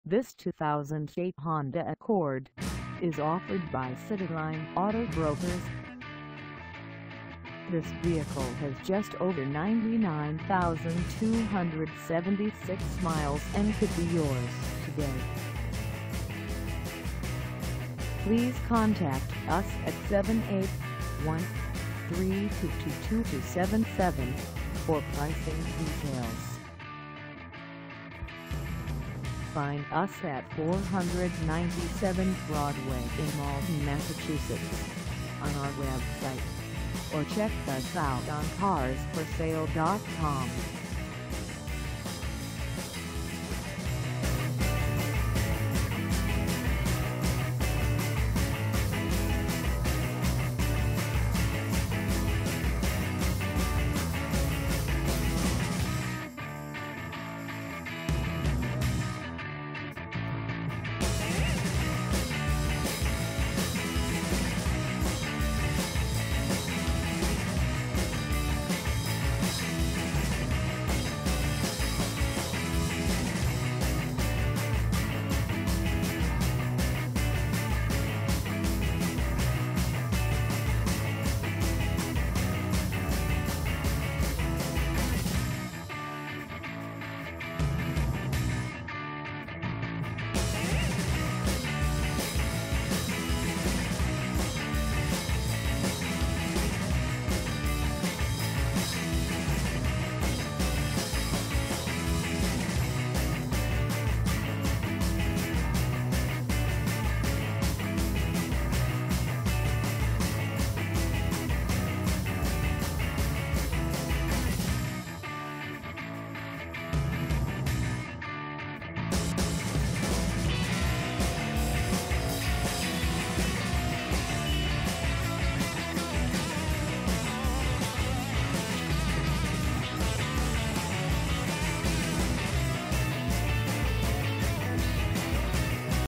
2008 Honda Accord LX 4dr Sedan 5A for sale in Malden, MA 021
This 2008 Honda Accord LX 4dr Sedan 5A is for sale in Malden, MA 02148 at Cityline Auto Brokers.

Engine: 2.4L I4
Transmission: Automatic 5-Speed
Color: GOLD
Mileage: 99276
Contact Cityline Auto Brokers 497 Broadway  Malden, MA 02148 to test drive this 2008 Honda Accord LX 4dr Sedan 5A today.